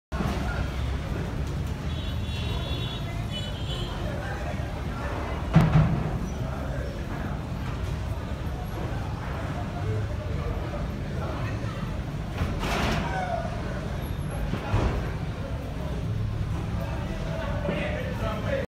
Attentat à Ouagadougou
Il est 10.05 juste après explosion du côté de l’état-major des armées - Ouagadougou